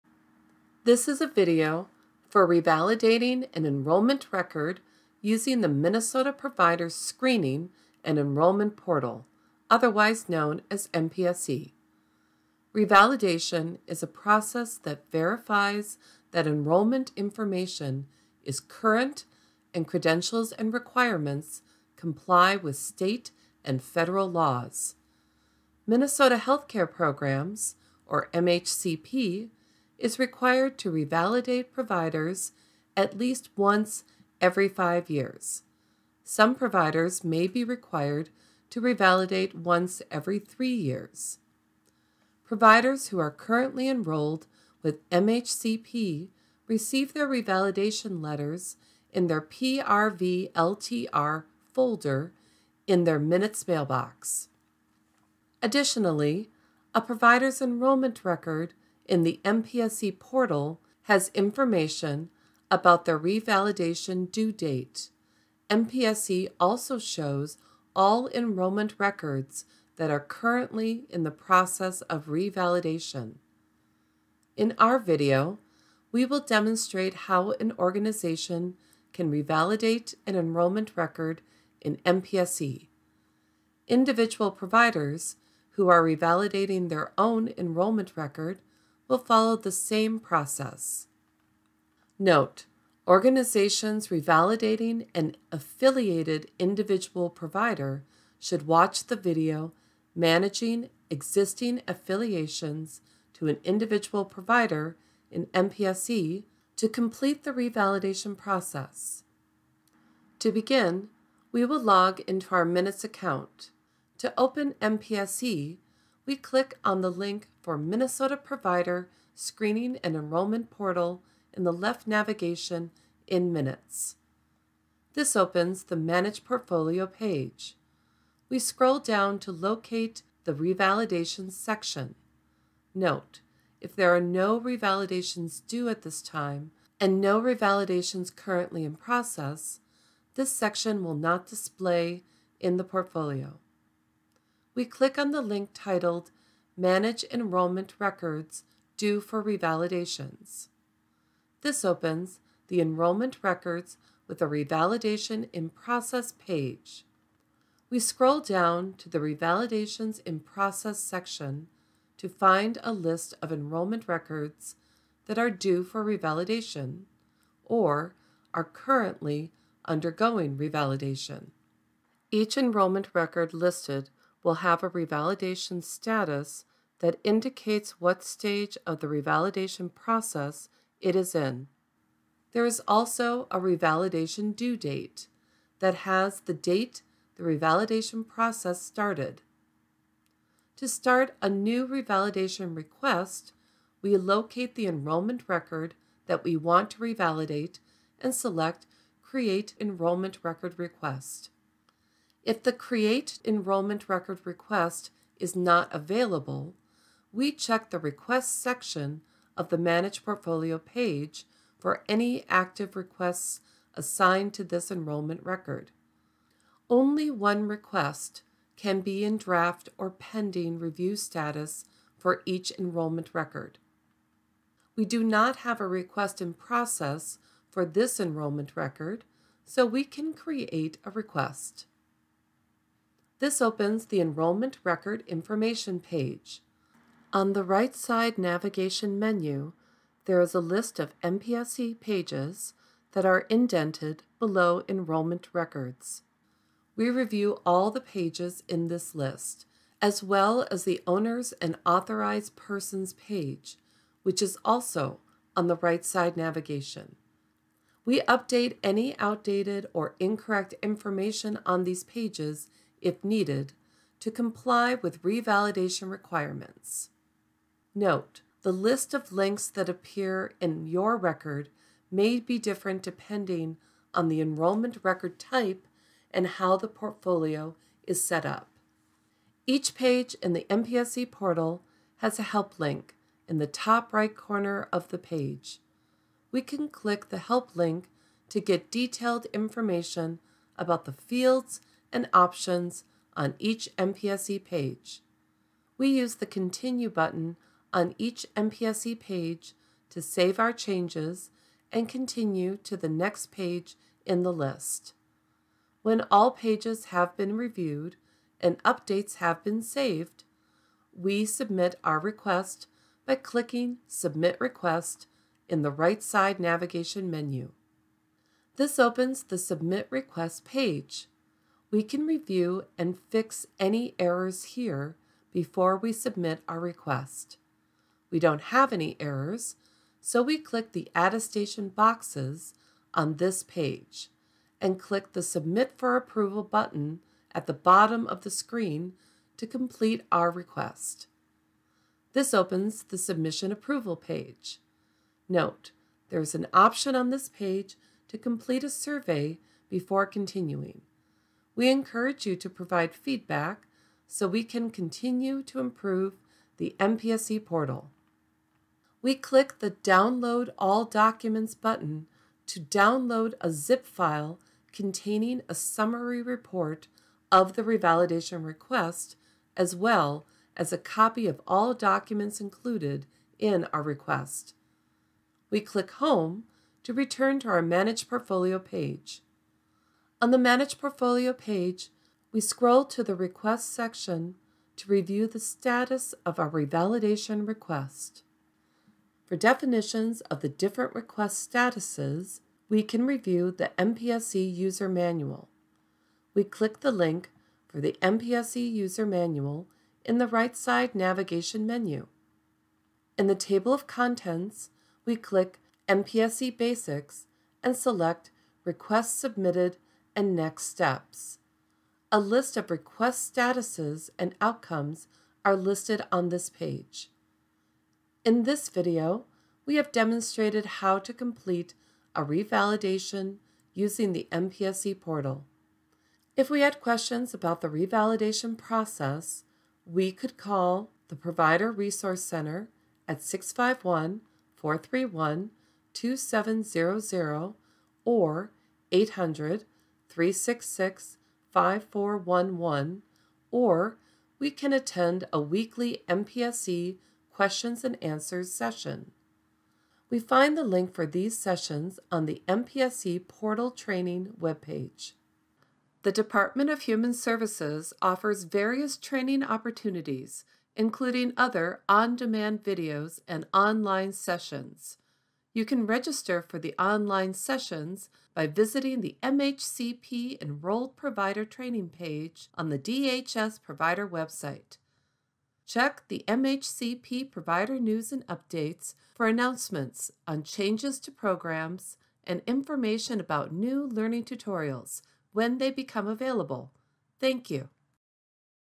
Revalidating an Enrollment Record Using MPSE
In our video, we will demonstrate how an organization can revalidate an enrollment record in MPSE. Individual providers who are revalidating their own enrollment record will follow the same process.